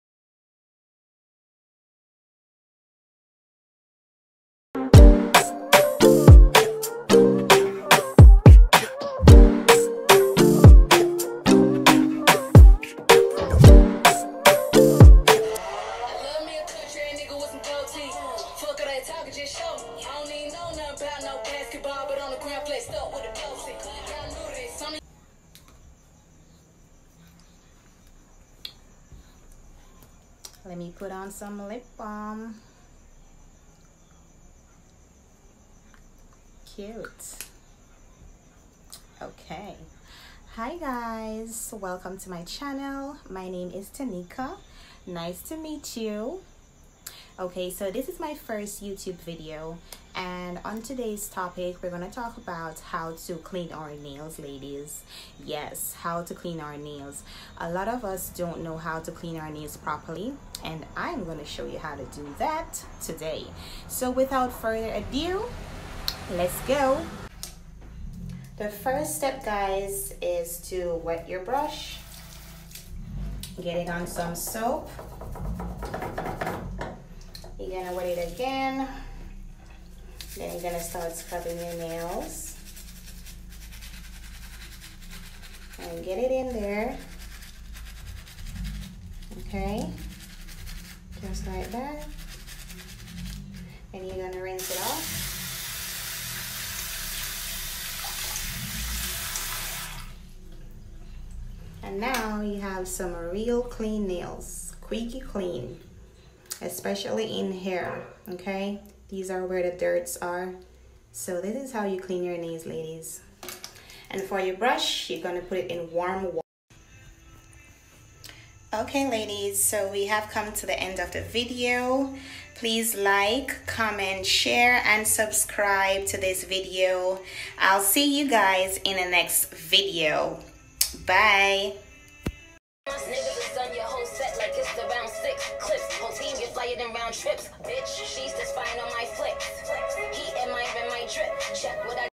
How to clean your nails at home|Thelovelytanika
How to clean nails at home.This video will help to show the proper way to clean your natural nails or acrylic tips.credit to Jeff kaale